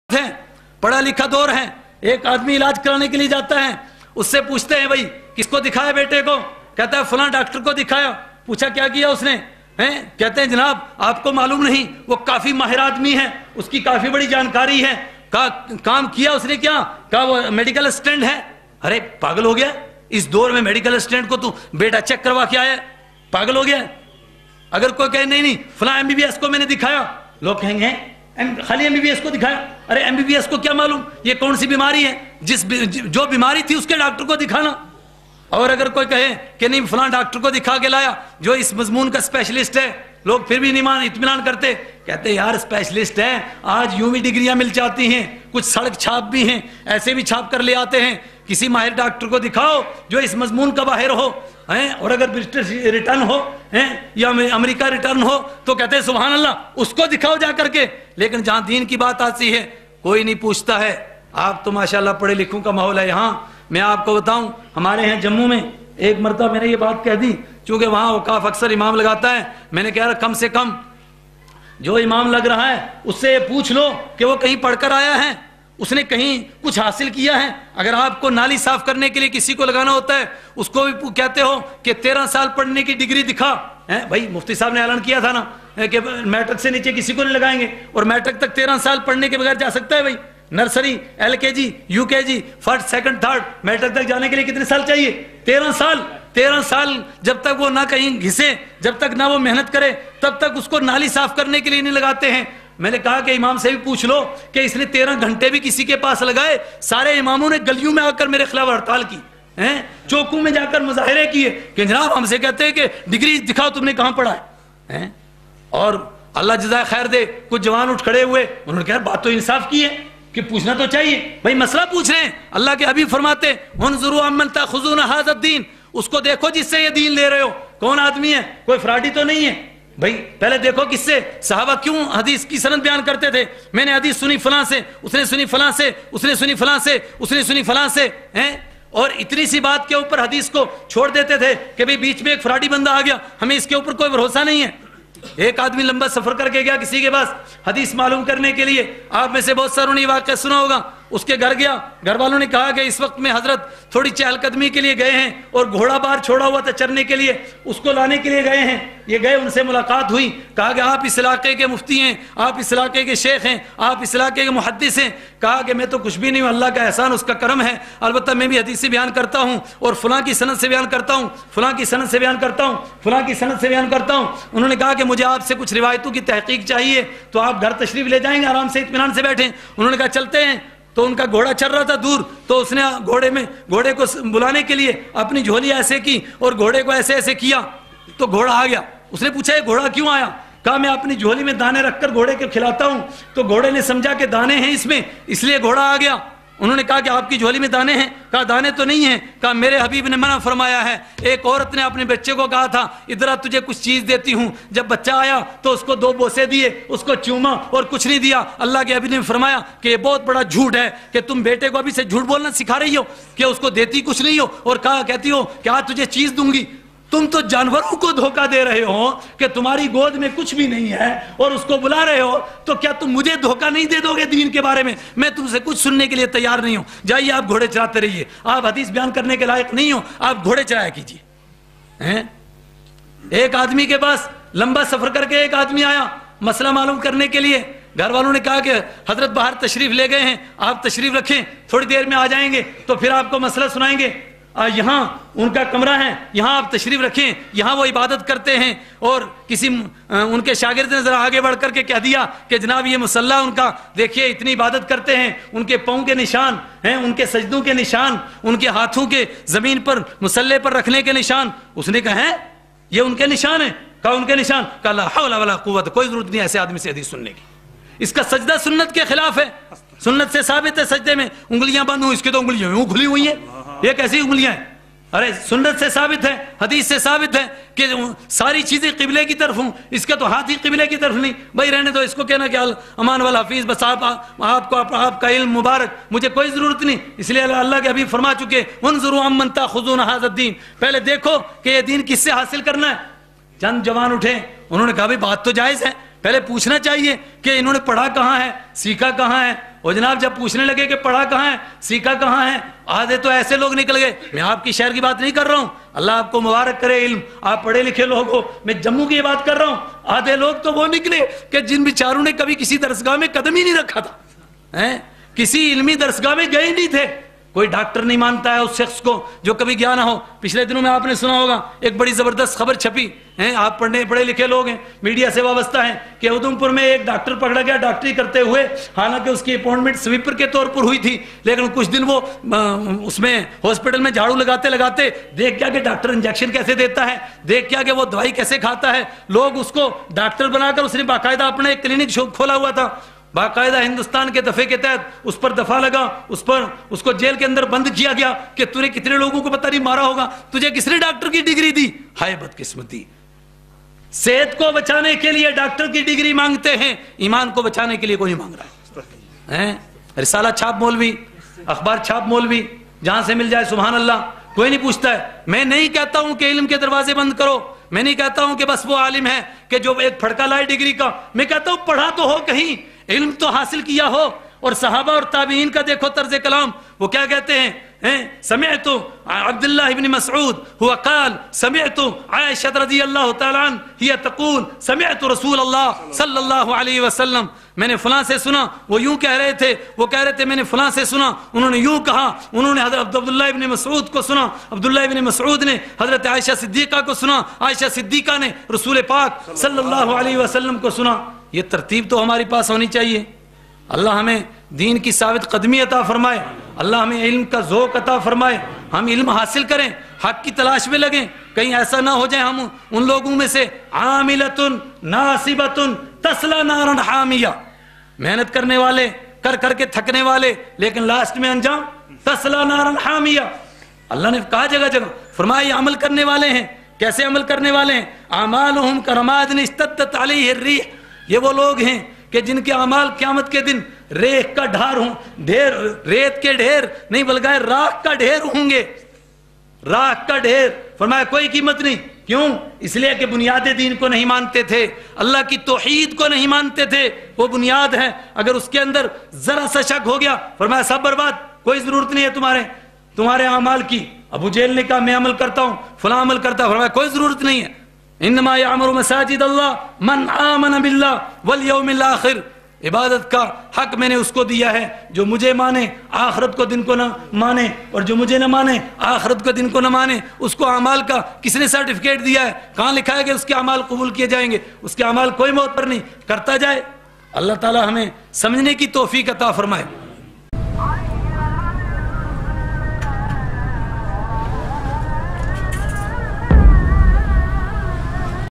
Sc03 Musalmanu Ky Pasty Ky Wajah-Mufti Faizul Waheed Qasmi(مسلمانو کی تنزلی کی واجہ)
مسلمانو کی تنزلی کی واجہ
سبب انحدار المسلمين
The reason for the decline of Muslims
मुसलमानों के पतन का कारण
মুসলমানদের পতনের কারণ